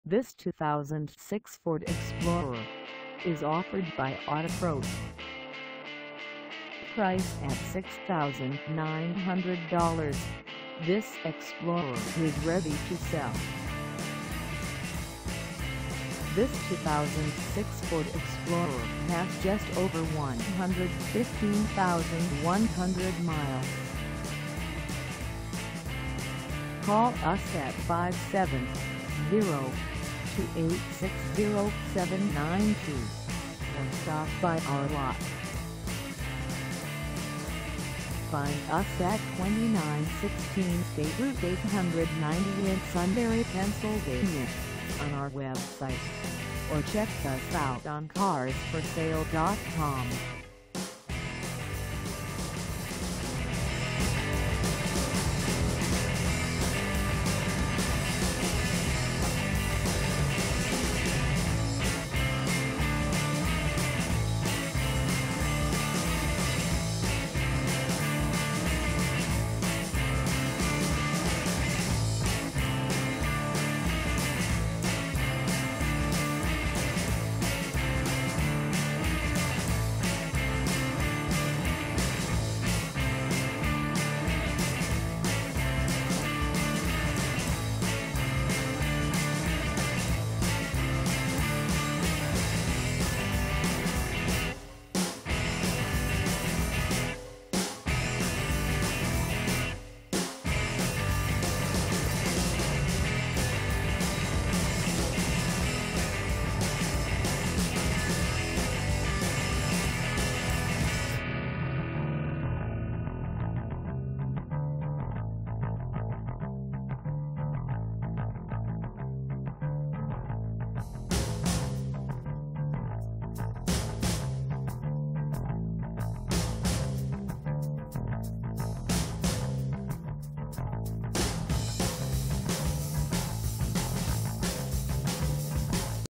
2006 Ford Explorer XLT 4dr SUV 4WD w/V6 for sale in Sunbury,
This 2006 Ford Explorer XLT 4dr SUV 4WD w/V6 is for sale in Sunbury, PA 17801 at Autopro.

Engine: 4.0L V6
Transmission: Automatic 5-Speed
Color: Red
Mileage: 115100
Contact Autopro 2916 State Route 890 Sunbury, PA 17801 to test drive this 2006 Ford Explorer XLT 4dr SUV 4WD w/V6 today.